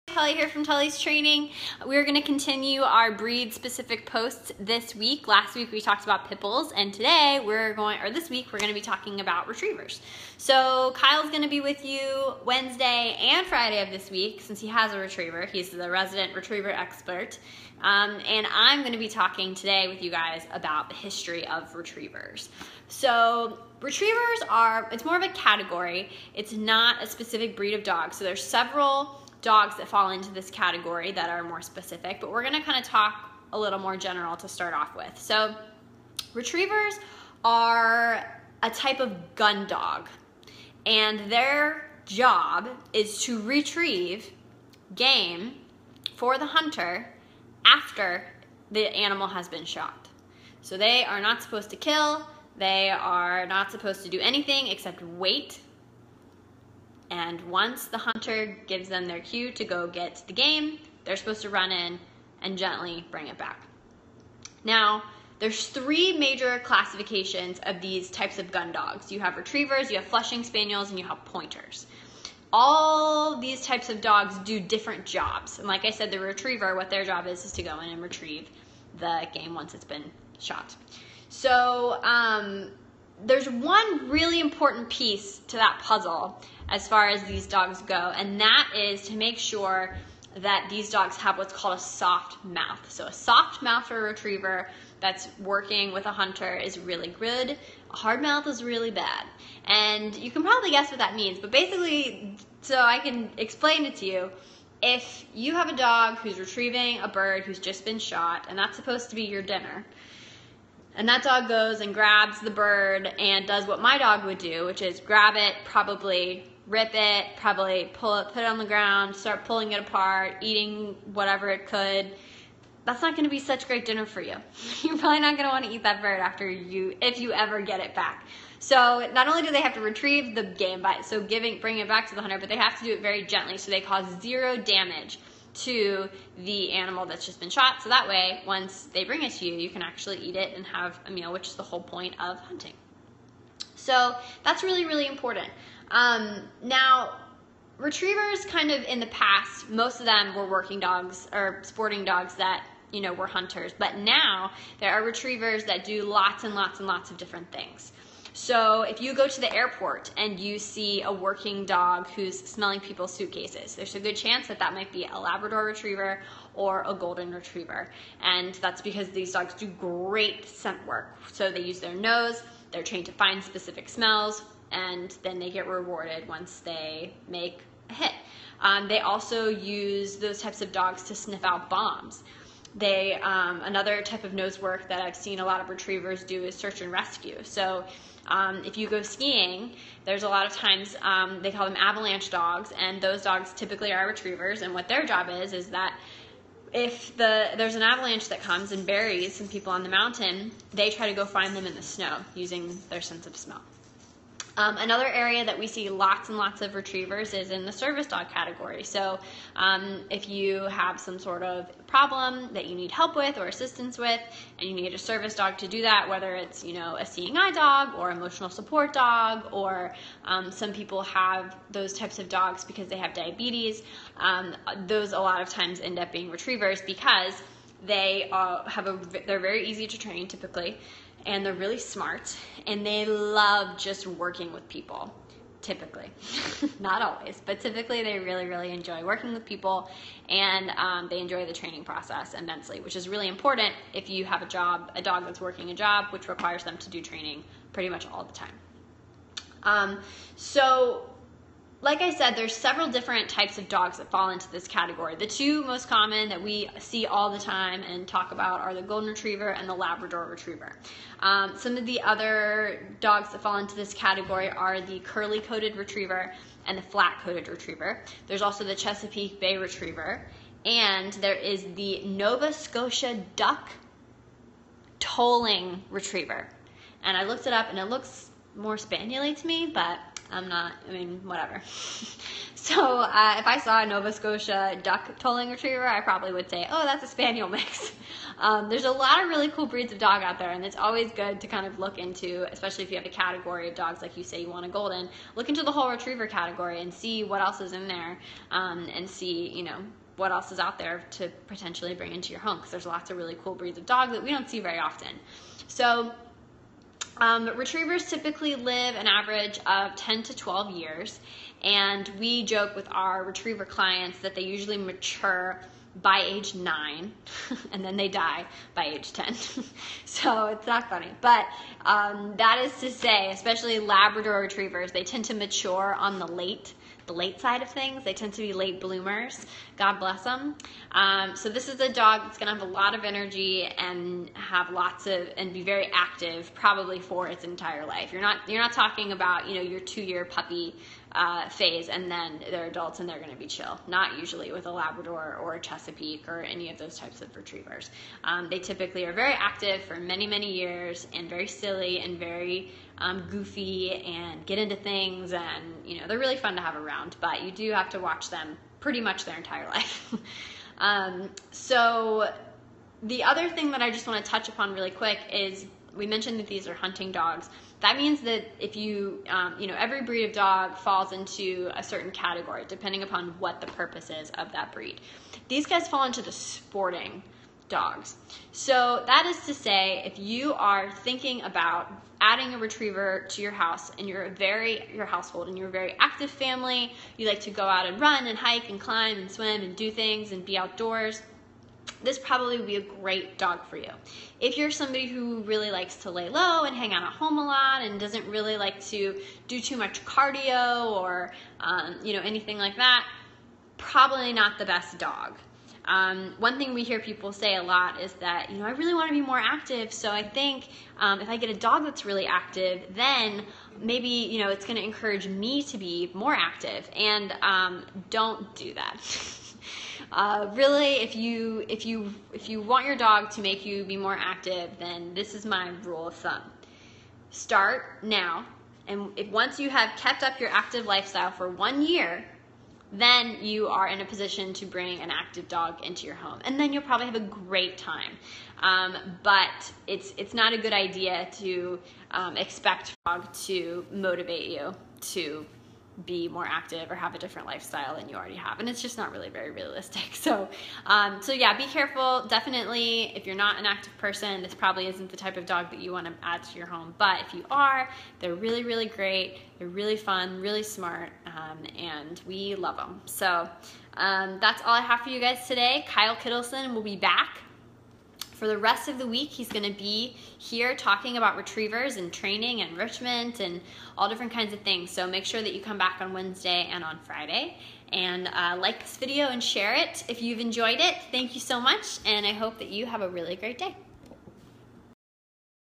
Tully’s Training Presents: Retriever History
Mary Tully teaches about the fascinating past of these sporting dogs – from retrieving game for hunters to current jobs like working in airports. Mary dive into history provides dog owners some insight into your retrievers present behaviors, that could be rooted from their past.

Want to learn more from, Mary Tully & Kyle Kittleson, or interested in having your dog trained? Check out Tully’s Training here: